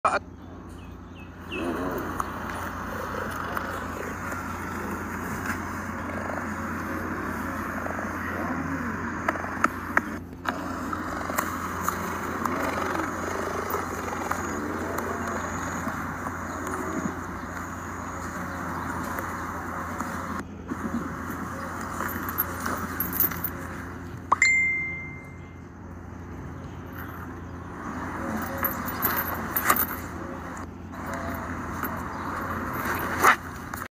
Beautiful lion, tigers, cheetah bigcats animals snake tiger lion zoo wildlife 🦁🦁🦁🦁🦁❤️❤️❤️
Beautiful lion tiger cheetah in ras al khaimah zoo.  available for photographs